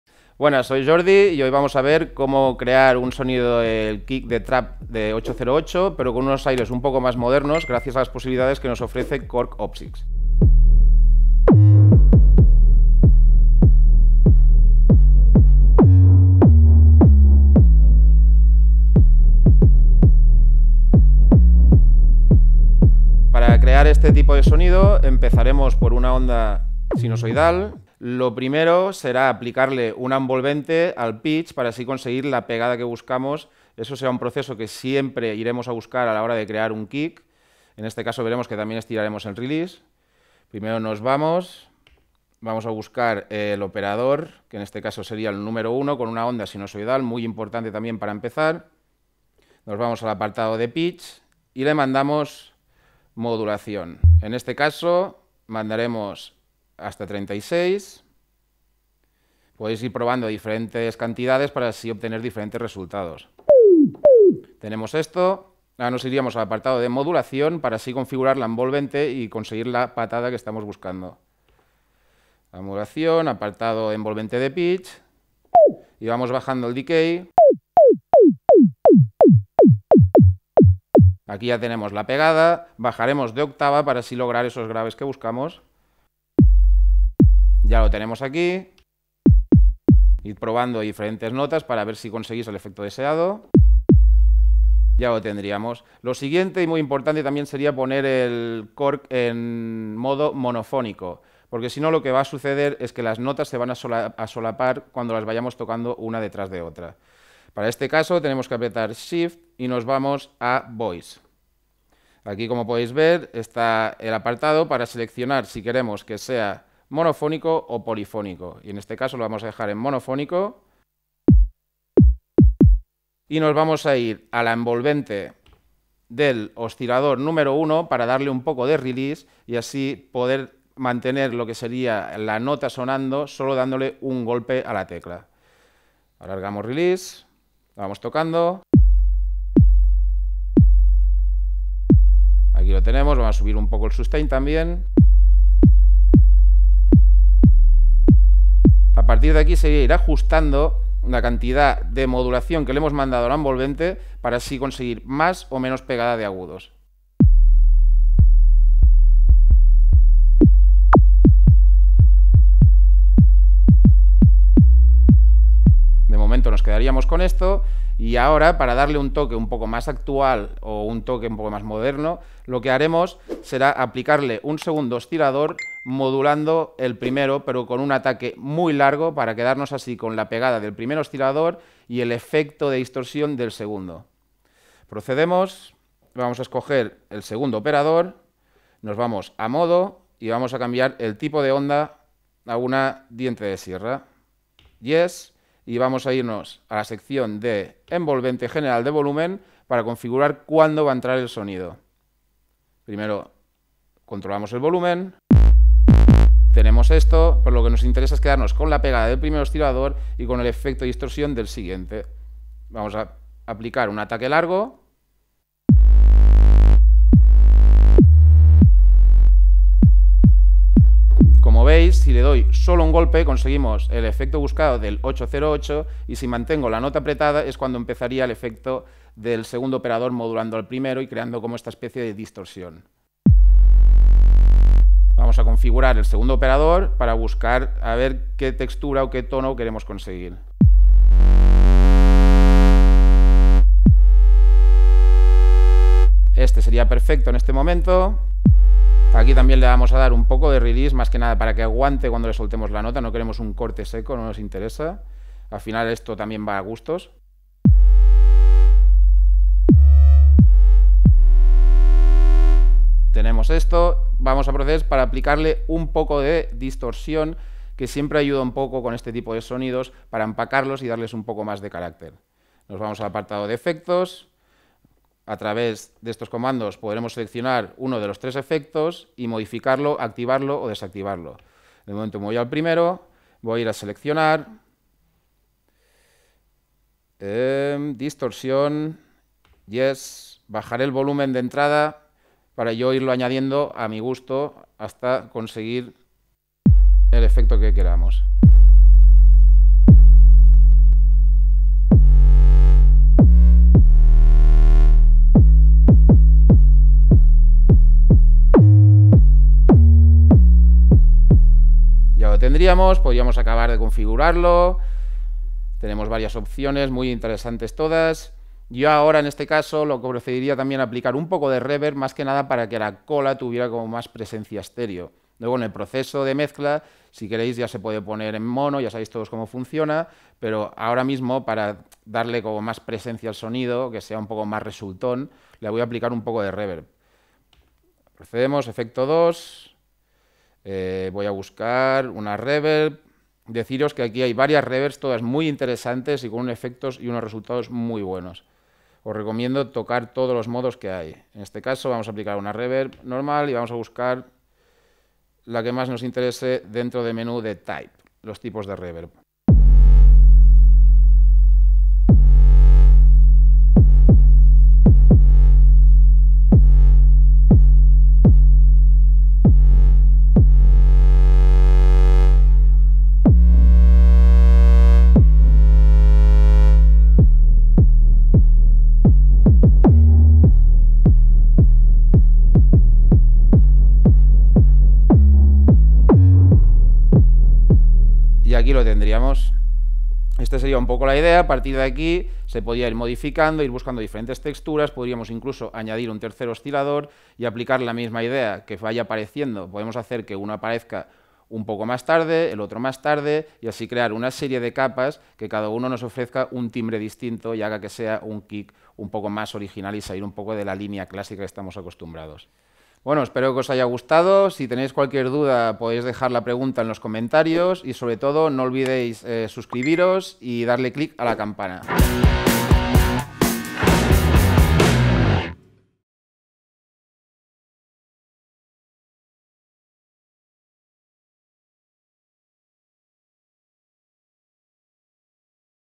KORG - Cómo crear un kick para TRAP estilo 808 con el nuevo sintetizador Opsix - Tutorial en español
El Productor, DJ y Diseñador de Sonido MAAE nos muestra cómo crear un Kick para Trap estilo 808 con un toque muy especial gracias a las posibilidades sonoras del nuevo sintetizador Korg opsix.  El opsix es un sintetizador FM "reinventado" de seis operadores. Con su mezclador de operador asombrosamente accesible y su motor de sonido flexible que va mucho más allá de la síntesis FM tradicional, este instrumento amplía al máximo el potencial de los sintetizadores digitales.